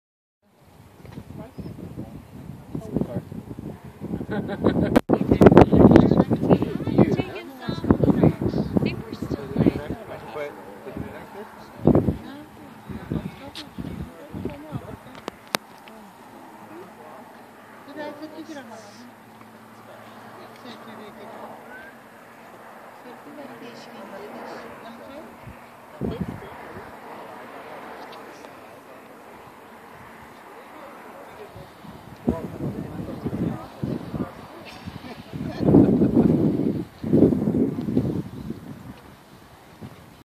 Highest Outlook 2018 Sept. Central Cascades
On the way home to Seattle we stopped to explore this outlook perched on the side of the mt. Near Diablo Dam and Ross Lake.